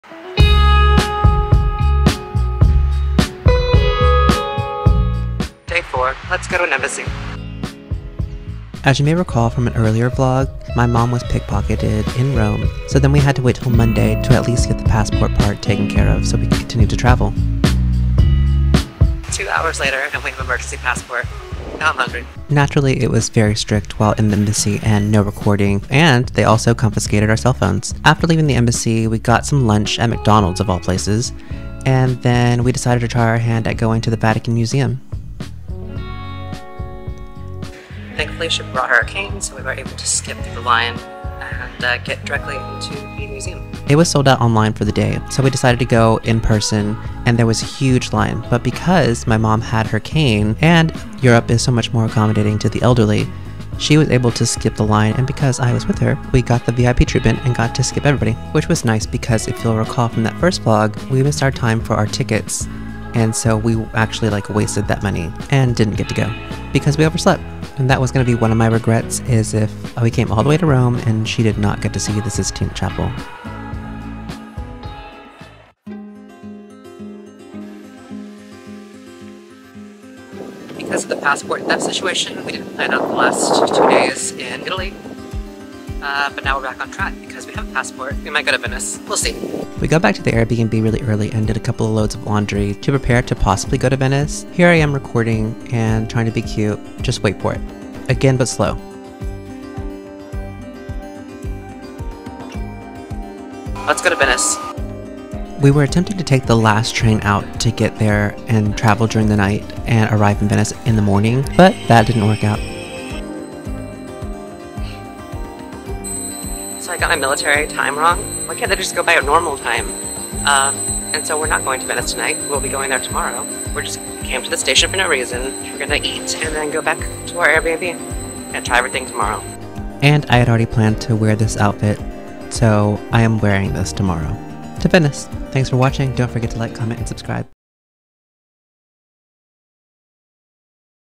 Day 4 in Rome: To The Embassy, Sistine Chapel, Vatican Museum & Wrong Train (ItalyVlog)
Day 4 in Rome: To The Embassy, Sistine Chapel, Vatican Museum & Wrong Train
(Italy Vlog)
Welcome back to my Europe trip vlog. This video is covering my 4th day in Italy. After
being pickpocketed we had to get to the Embassy to get new passports and we
managed to visit the Vatican Museum and the Sistine Chapel, so it was a fun and
busy day. We booked train tickets to go to Venice, but we didn't get the hours right,
so that's a trip for the next day.